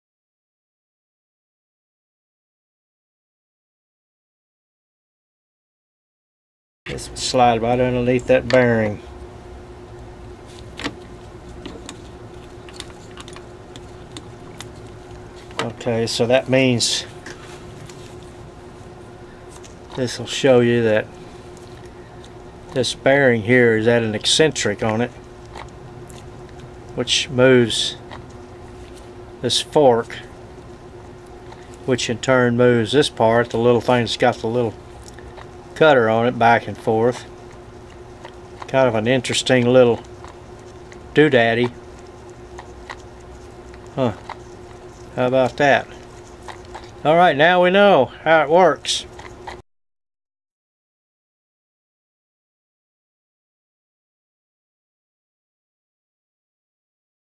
multi-tool and how it works
Took a Worx multi tool apart to repair it and wanted to show how it moves the cutting tool back and forth